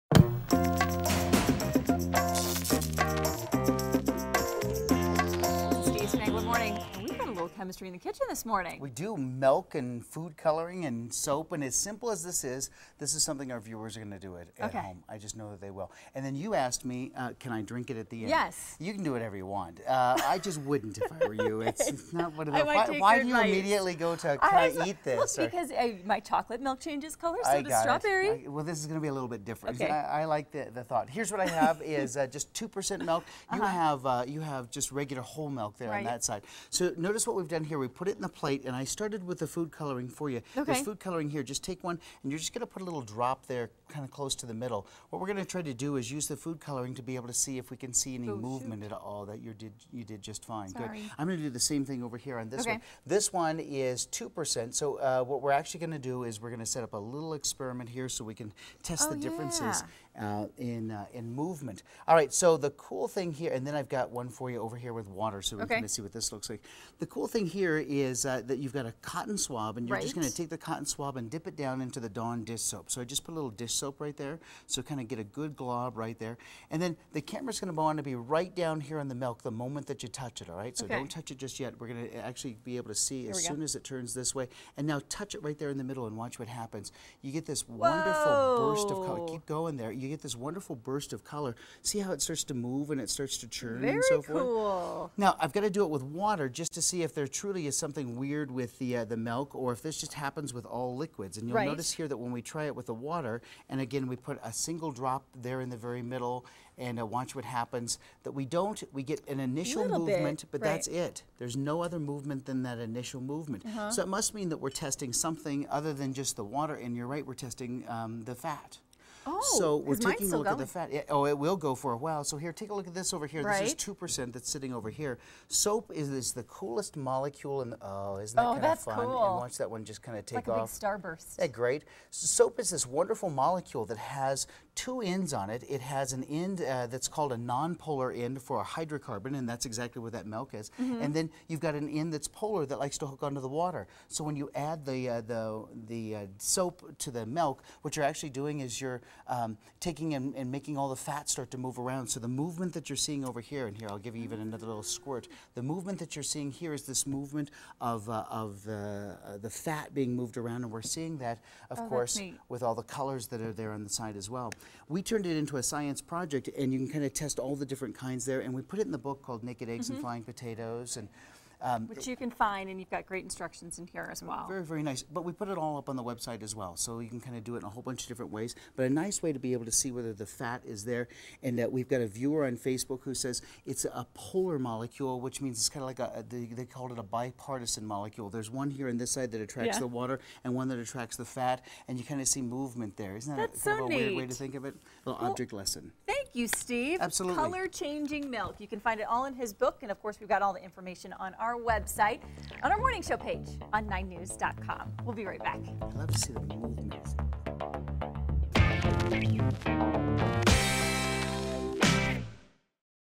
Color Changing Milk - Cool Science Experiment
It's an explosion of color! Some very unusual things happen when you mix a little milk, food coloring, and a drop of liquid soap. Use this experiment to amaze your friends and uncover the scientific secrets of soap. About Steve Spangler Science...